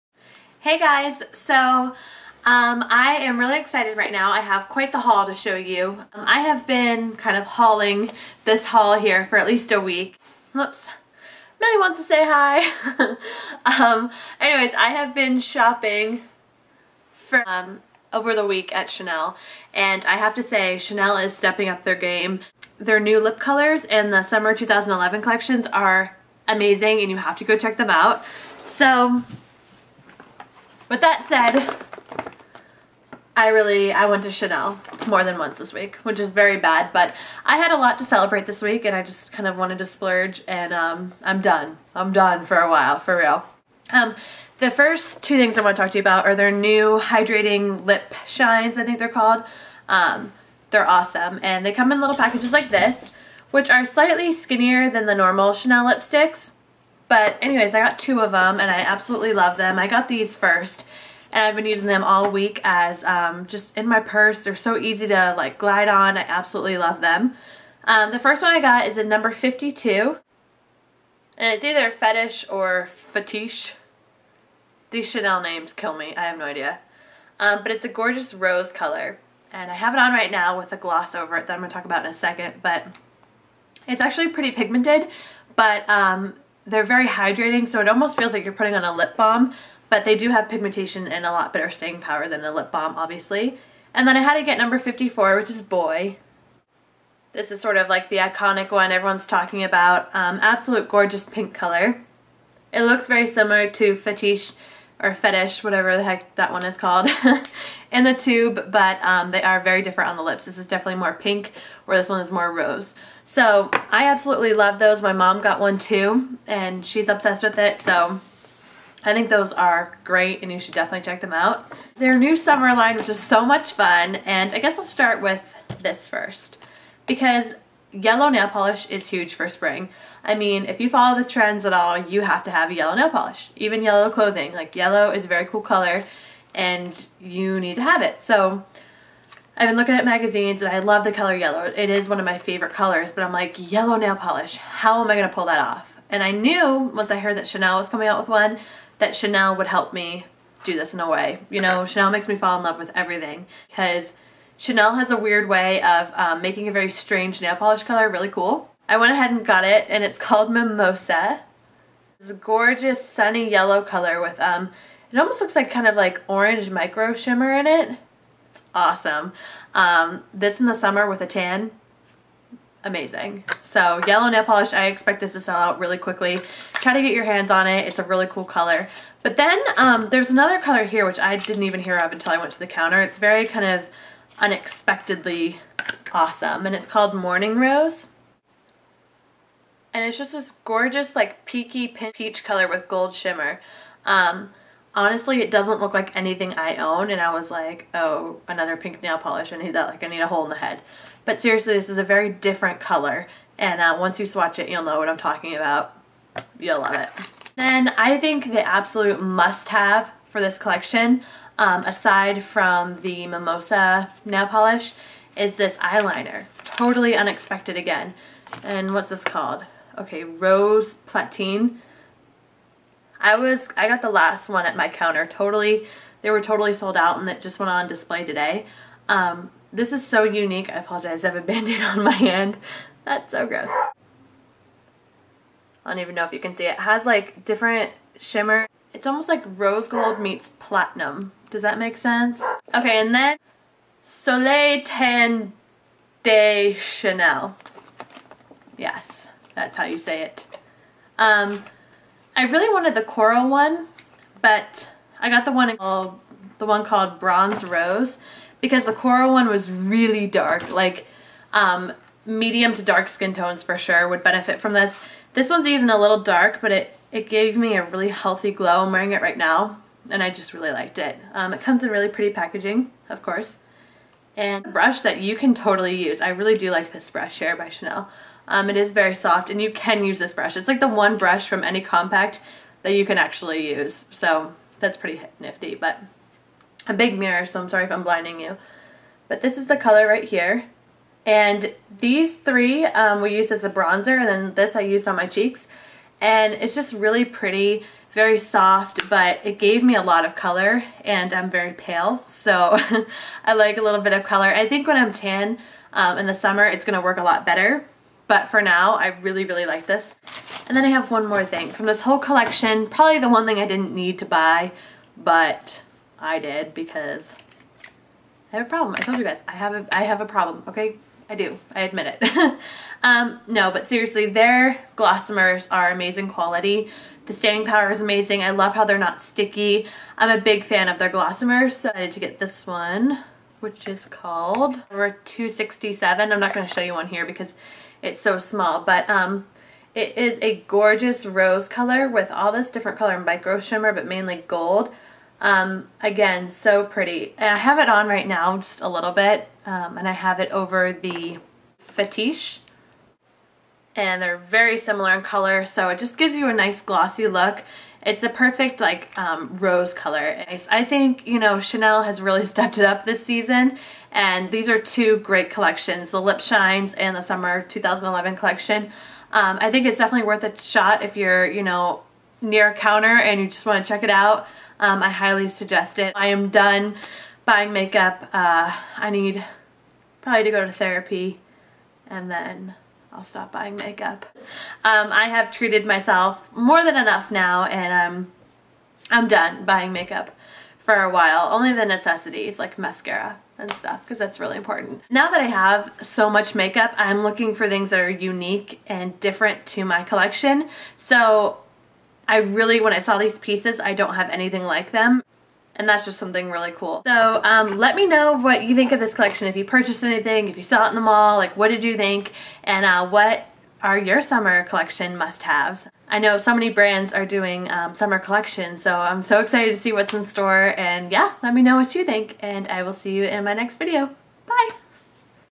Chanel Summer 2011 makeup haul
This is a haul from the new Chanel makeup collections! Both are available now! Hope you enjoy!